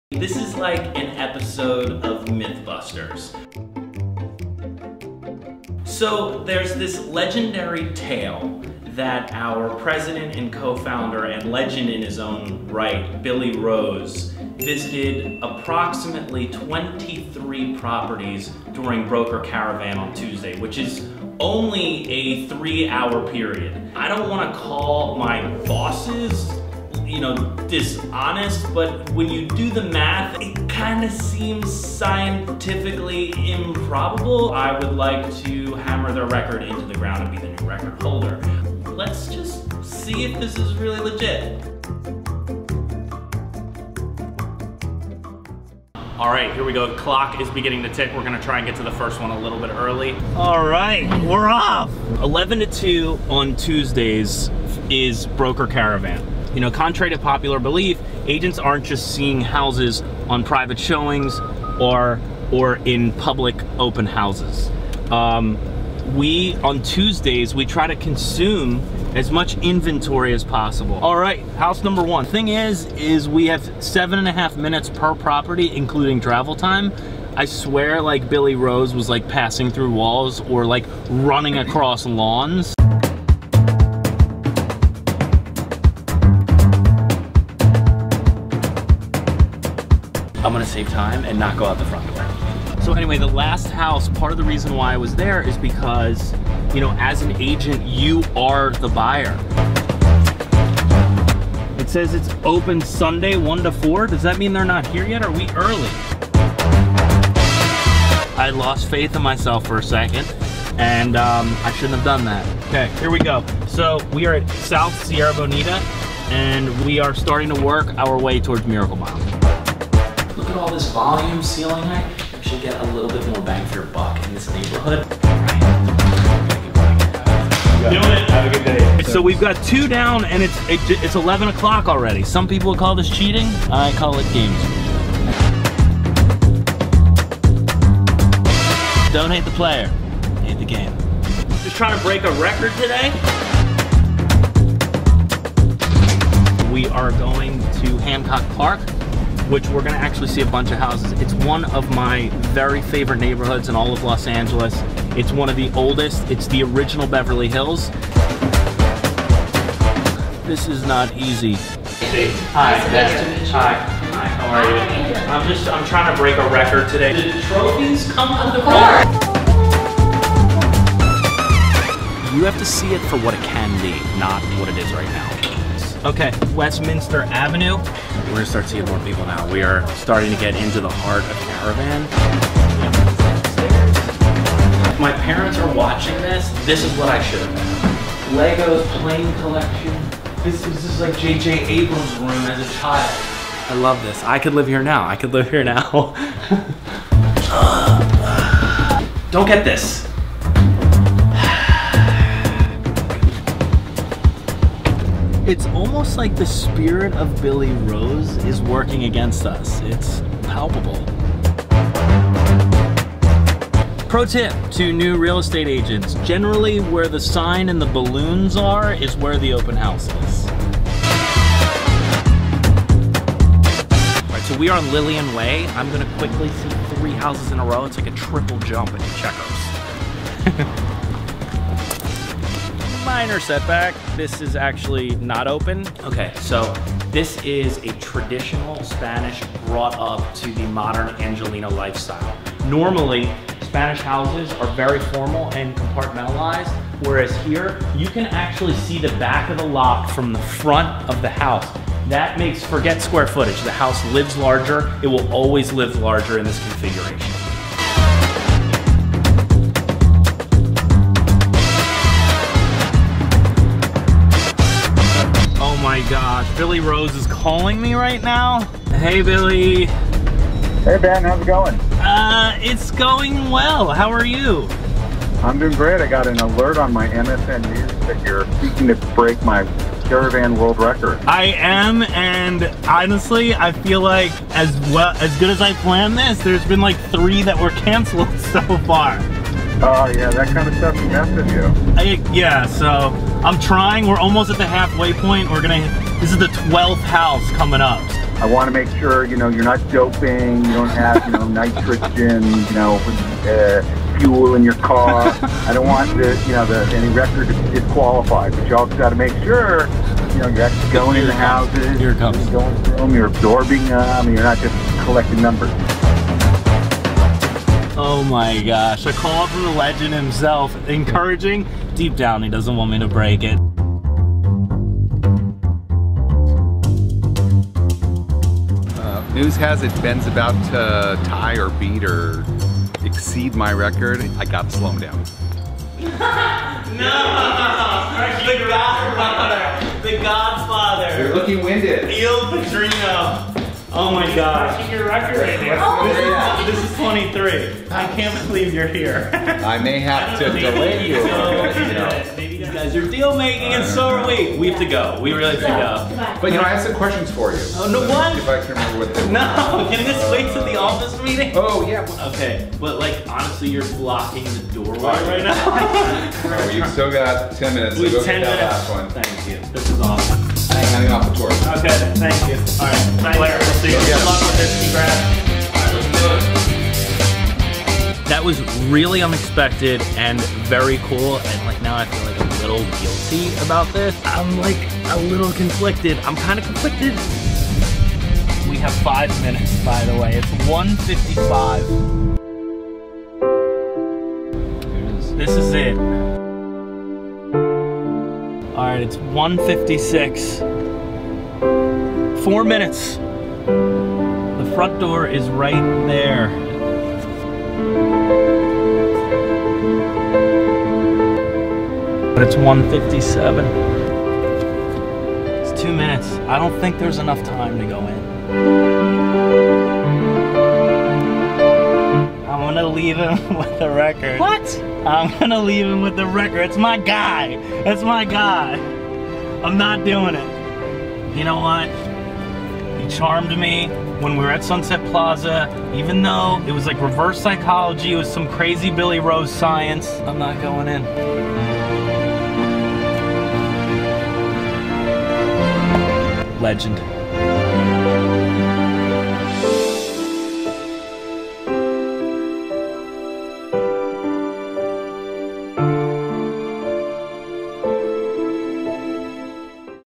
Beverly Hills Realtor Visits 23 Houses in 180 Minutes!
On Tuesdays in Los Angeles, real estate brokers see new inventory from 11am-2pm during Broker Open Houses (commonly known as Broker Caravan). Billy Rose’s Broker Caravan record is a legend amongst realtors nationwide - an insane 23 houses.  That leaves less than 8 minutes per house, INCLUDING TRAVEL TIME. Visiting 23 open houses in three hours is quite a feat, and I set out to see if it can actually be done!

Looking to sell your home?

How much is your home worth?